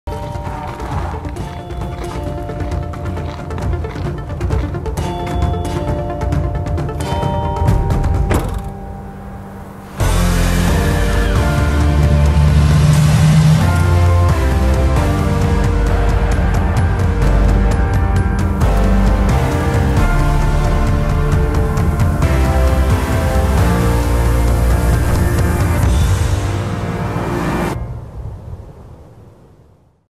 Stormberg of Norway
My exams project in commercial photography for Stormberg of Norway